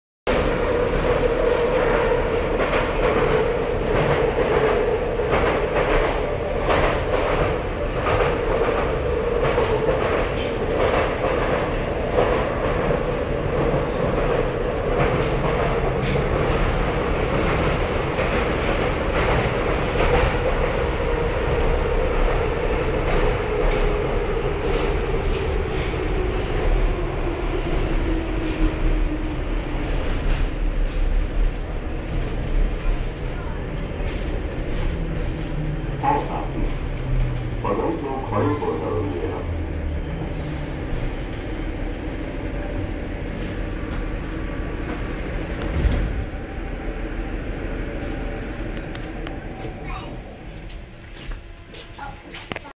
Budapest Metró 2010.06.13
Ev3 - 254 in Budapest, Forgách utca - Árpád híd, metro line M3, 8:36 PM.